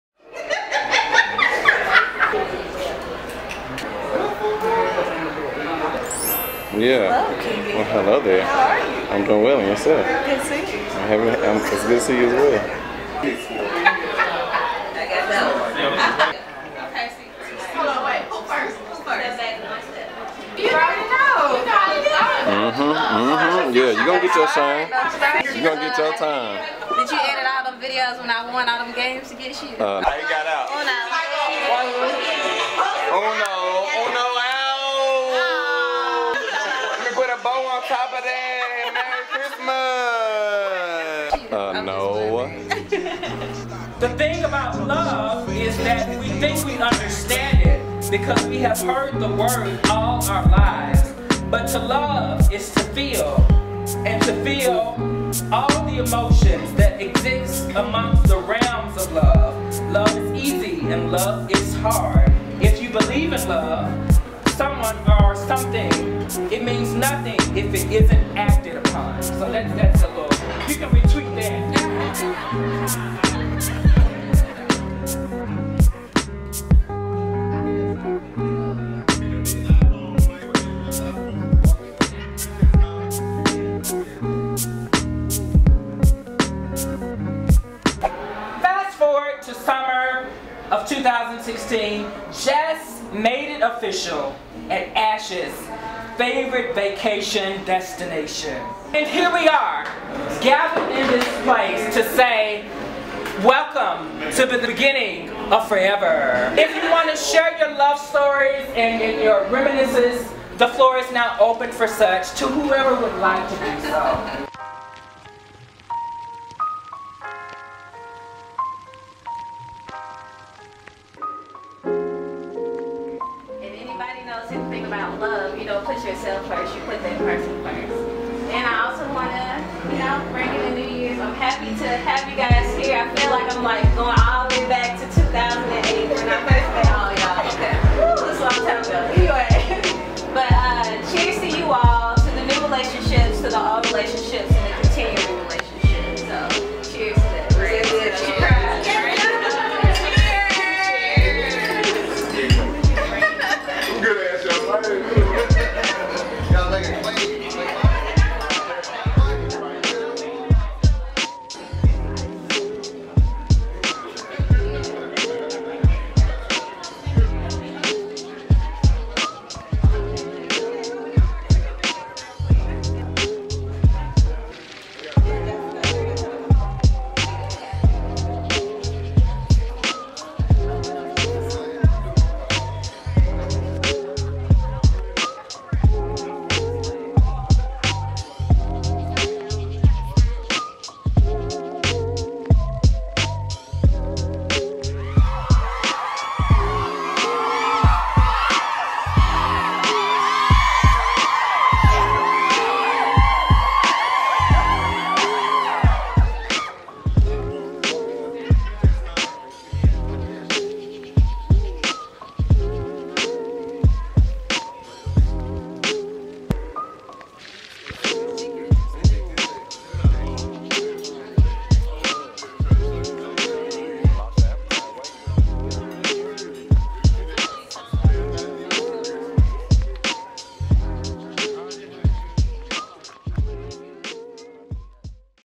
The Noxolo Engagement Dinner
December 31, 2016, the friends and family of Ashley and Jessica, joined to celebrate the upcoming September nuptials of the couple.